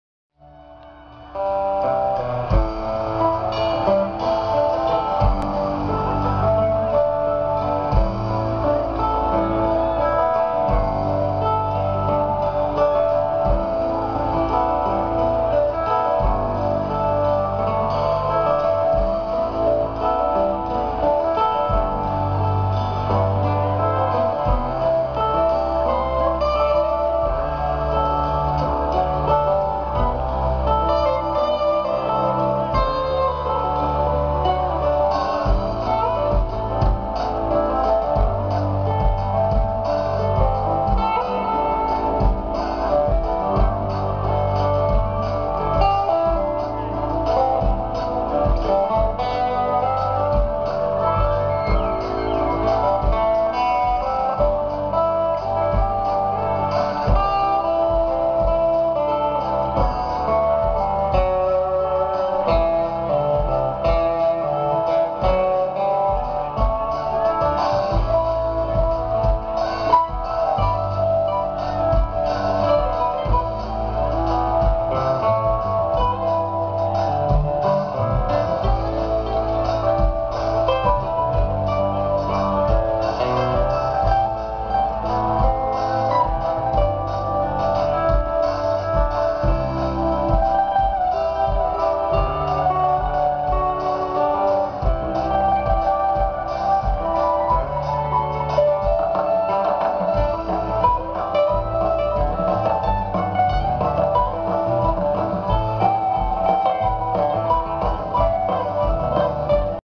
Camille Performing @ YMSB's Harvest Festival
Camille performs at Yonder Mountain String Band's Harvest Music Festival @ Mulberry Mountain in Ozark, AR  in October during Railroad Earth!

Camille Rule
-Hoop Dancer- Fire Dancer- Movement Artist-